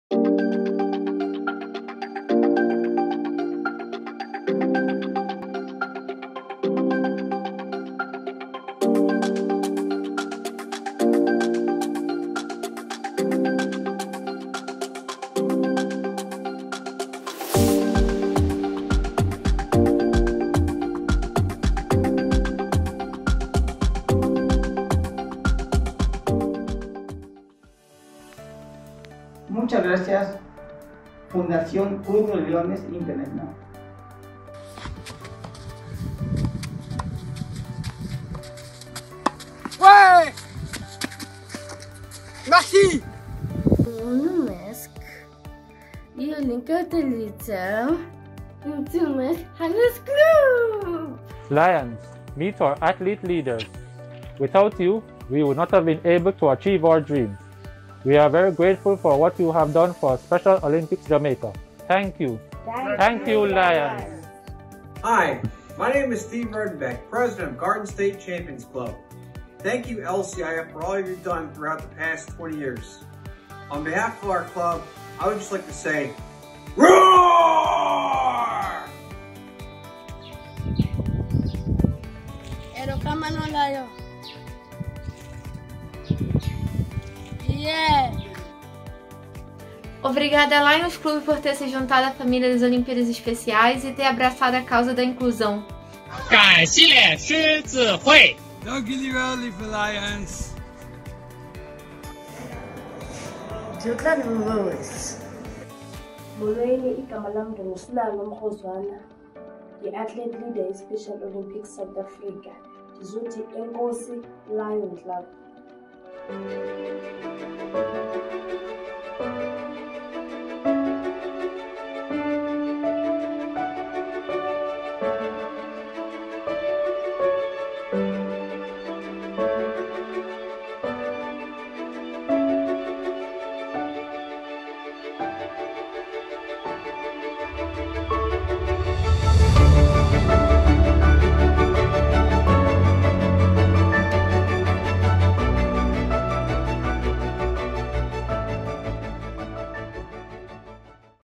Thank you Lions Clubs International Foundation!
For 20 years, Lions and Leos volunteers have been working with Special Olympics to help create a truly inclusive world through sports & service. From Brazil to Belgium and from Mexico to South Africa, the athletes of Special Olympics have a simple message: Thank you Lions Clubs International Foundation!